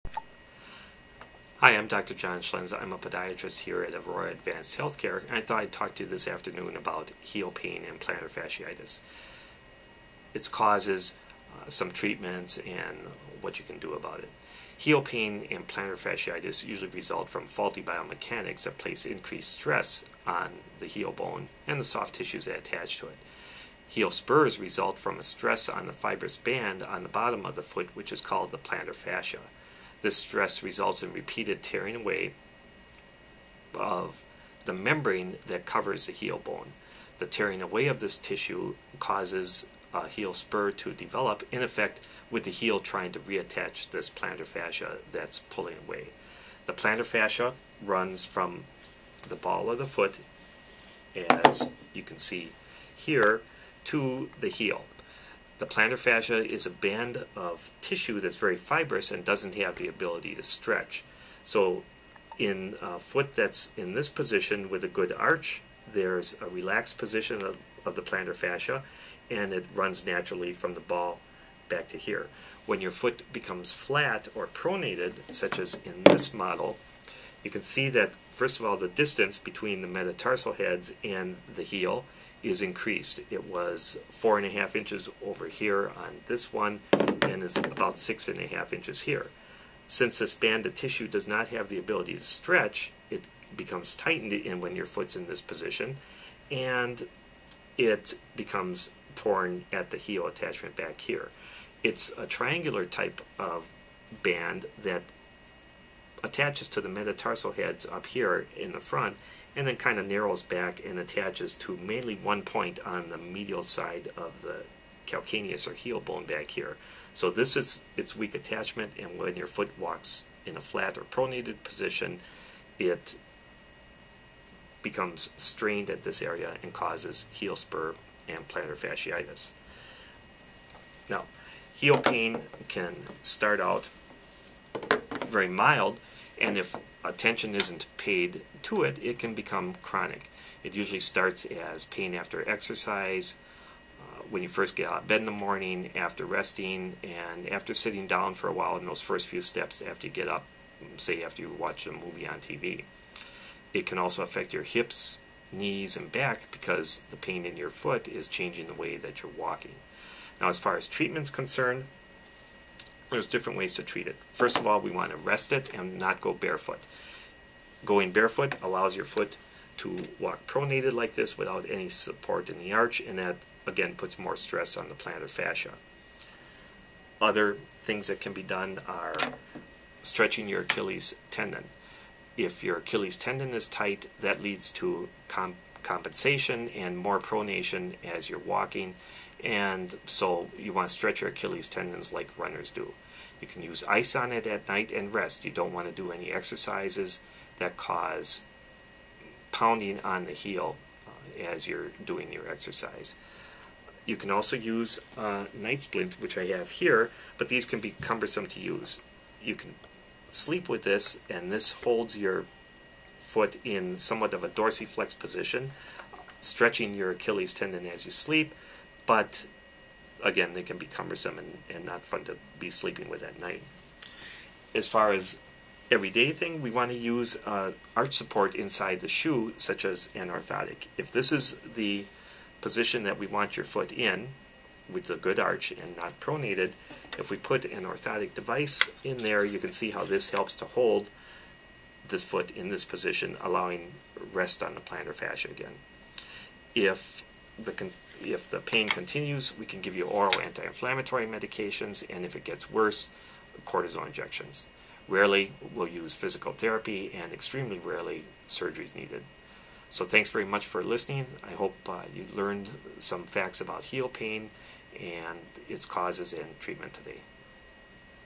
Plantar Fasciitis | Heel Pain | Dr. John Schlenz | Orthopaedics | South East Wisconsin
This is a video of Dr. John Schlenz explaining the facts of plantar fasciitis.  What causes plantar fasciitis, heel pain.  How can you prevent plantar fasciitis.  Conservative treatment of inflammation of the plantar fascia.